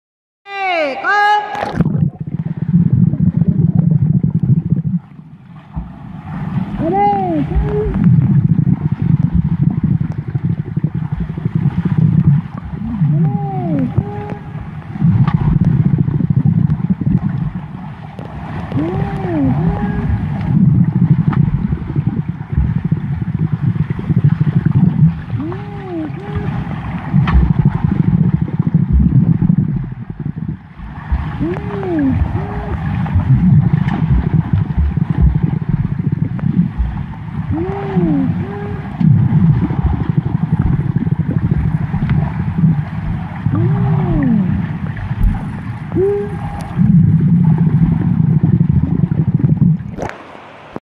문화관 새벽 수영 잠영 영상
현대기아문화관 새벽 6시반 스타트 잠영 영상